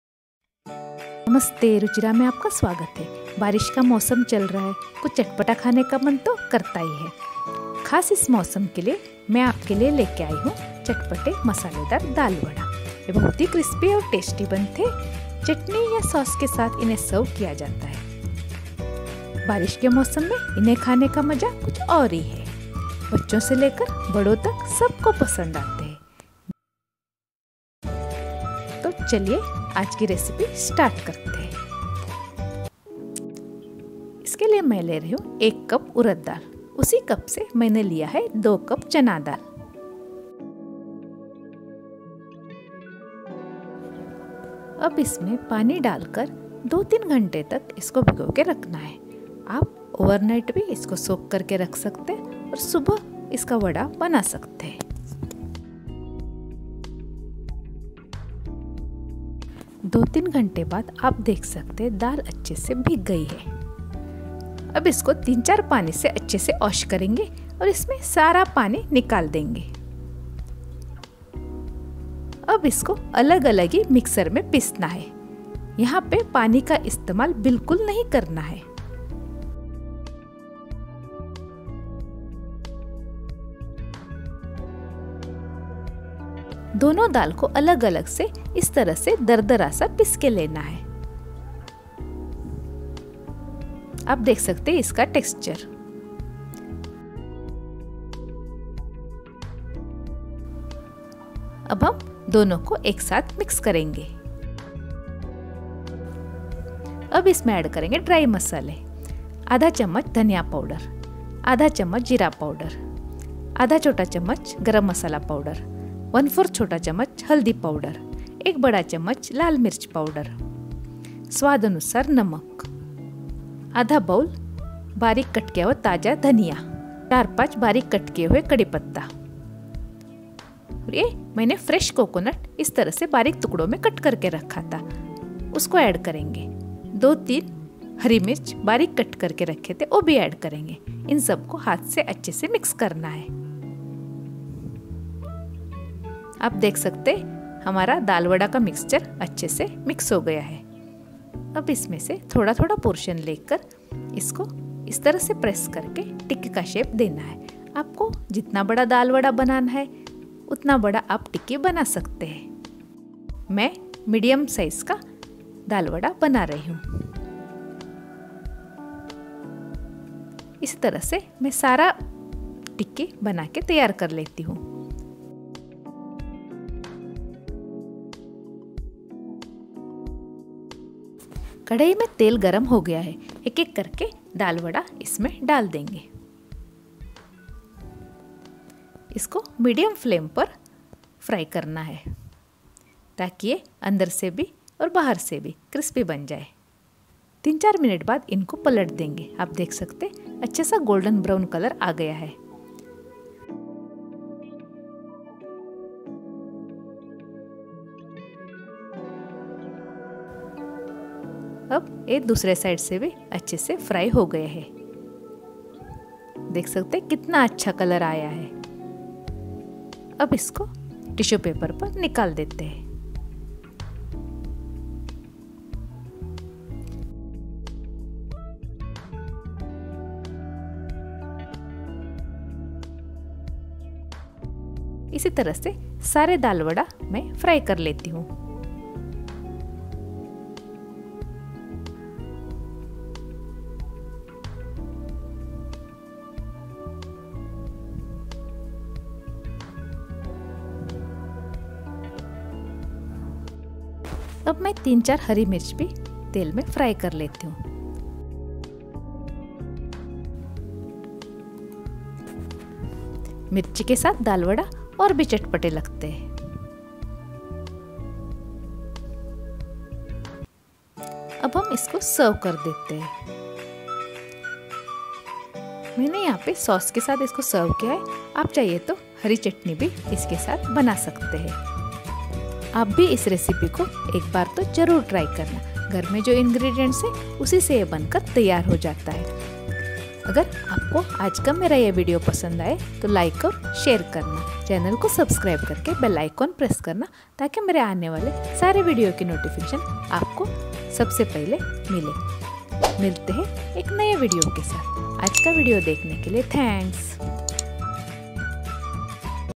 Dal vada | Monsoon special crispy dal vada recipe | Masala vada | Ruchira
Chana dal -2 cups
Fresh coriander leaves(finely chopped) -1/2 cup
Fresh coconut(cut into small pieces) -2 tbsp